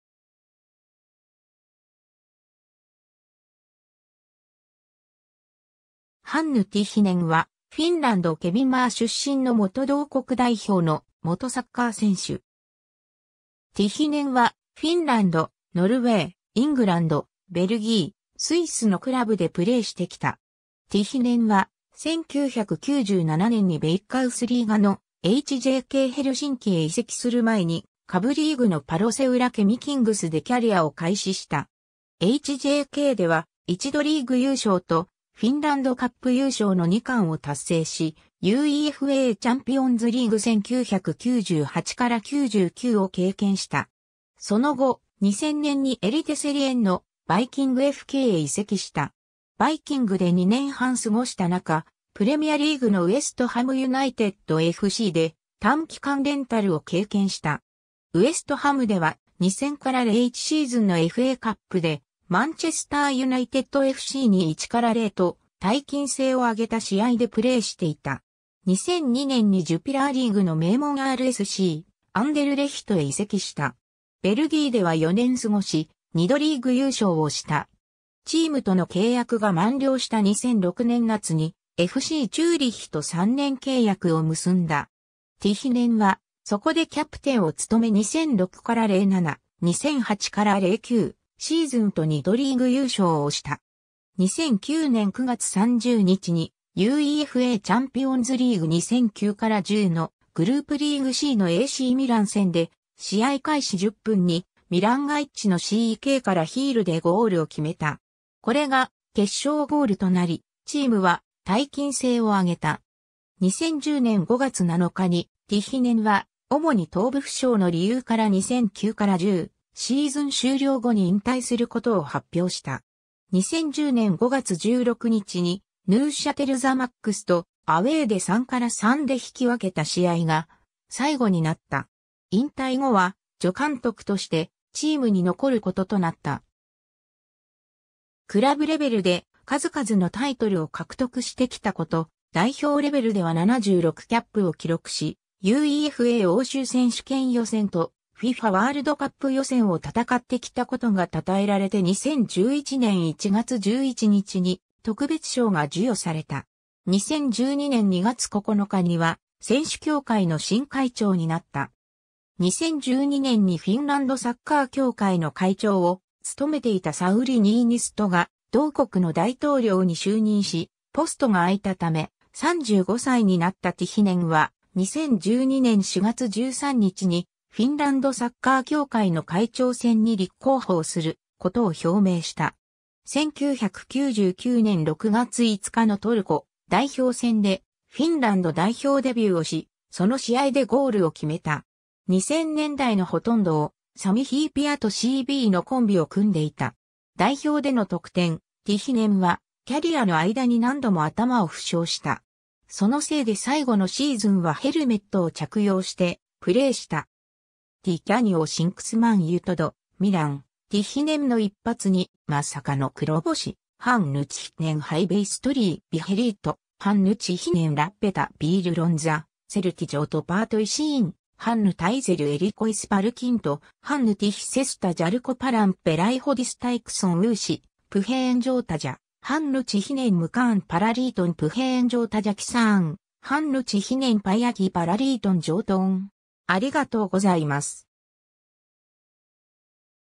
ハンヌ・ティヒネン
パロセウラ・ケミ・キングスの選手
ハンヌ・ティヒネン（Hannu Tihinen, 1976年7月1日 - ）は、フィンランド・ケミンマー出身の元同国代表の元サッカー選手。
 ティヒネンは、フィンランド, ノルウェー, イングランド, ベルギー, スイスのクラブでプレーしてきた。
 ティヒネンは、1997年にヴェイッカウスリーガのHJKヘルシンキへ移籍する前に下部リーグのパロセウラ・ケミ・キングスでキャリアを開始した。
HJKでは、1度リーグ優勝とフィンランドカップ優勝の2冠を達成し、UEFAチャンピオンズリーグ 1998-99を経験した。
その後、2000年にエリテセリエンのバイキングFKへ移籍した。
バイキングで2年半過ごした中、プレミアリーグのウェストハム・ユナイテッドFCで短期間レンタルを経験した。
ウェストハムでは、2000-01シーズンのFAカップでマンチェスター・ユナイテッドFCに1-0と大金星を挙げた試合でプレーしていた。
 2002年にジュピラーリーグの名門RSCアンデルレヒトへ移籍した。
ベルギーでは4年過ごし、2度リーグ優勝をした。
チームとの契約が満了した2006年夏に、FCチューリッヒと3年契約を結んだ。
ティヒネンは、そこでキャプテンを務め2006-07, 2008-09シーズンと2度リーグ優勝をした。
 2009年9月30日に、UEFAチャンピオンズリーグ 2009-10のグループリーグCのACミラン戦で試合開始10分にミラン・ガイッチのCKからヒールでゴールを決めた。
これが決勝ゴールとなり、チームは大金星を挙げた。
 2010年5月7日に、ティヒネンは主に頭部負傷の理由から2009-10シーズン終了後に引退することを発表した。
2010年5月16日に、ヌーシャテル・ザマックスとアウェーで3-3で引き分けた試合が最後になった。
 引退後は助監督としてチームに残ることとなった。
 クラブレベルで数々のタイトルを獲得してきたこと、代表レベルでは76キャップを記録し、UEFA欧州選手権予選とFIFAワールドカップ予選を戦ってきたことが讃えられて2011年1月11日に、特別賞が授与された。
 2012年2月9日には、選手協会の新会長になった。
 2012年にフィンランドサッカー協会の会長を務めていたサウリ・ニーニストが同国の大統領に就任しポストが空いたため、35歳になったティヒネンは、2012年4月13日にフィンランドサッカー協会の会長戦に立候補をすることを表明した。
 1999年6月5日のトルコ代表戦でフィンランド代表デビューをし、その試合でゴールを決めた。
2000年代の殆どをサミ・ヒーピアとCBのコンビを組んでいた。
 代表での得点 ティヒネンは、キャリアの間に何度も頭を負傷した。
そのせいで最後のシーズンはヘルメットを着用してプレーした。
 ^ "Di Canio sinks Man Utd" ^ ミラン、ティヒネンの一発にまさかの黒星 ^ "Hannu Tihinen hyvästeli viheriöt" ^ "Hannu Tihinen lopettaa peliuransa - siirtyy johtoportaaseen" ^ "Hannu Tihiselle erikoispalkinto" ^ "Hannu Tihisestä jalkapallon Pelaajayhdistyksen uusi puheenjohtaja" ^ "Hannu Tihinen mukaan Palloliiton puheenjohtajakisaan" ^ "Hannu Tihinen pyrkii Palloliiton johtoon"